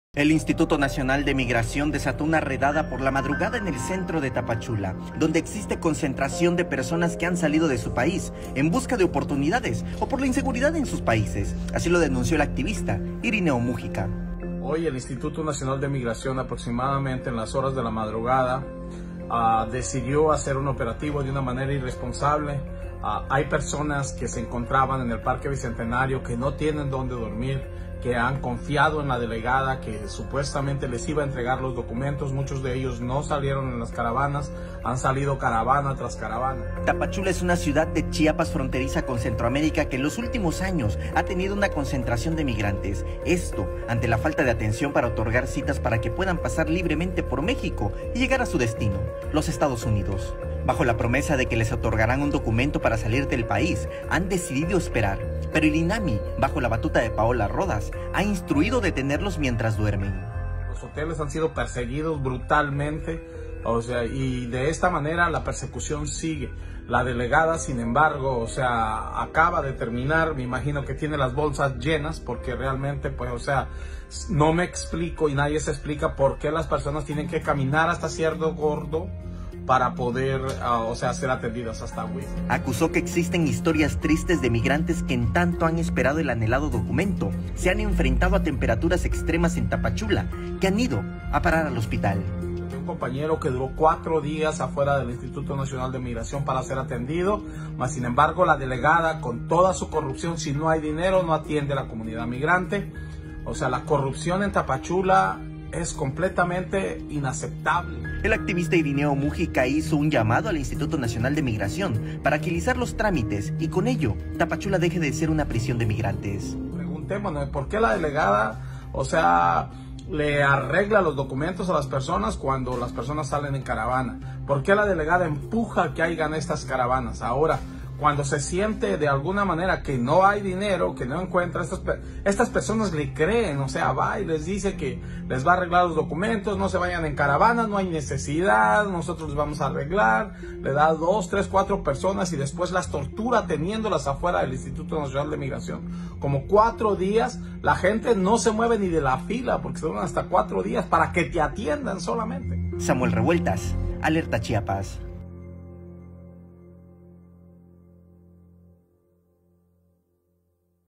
Migración desata redada en la madrugada en Tapachula
El Instituto Nacional de Migración desató una redada por la madrugada en el centro de Tapachula, donde existe concentración personas que han salido de su país en busca de oportunidades o por la inseguridad en sus países, denunció el activista Irineo Mújica.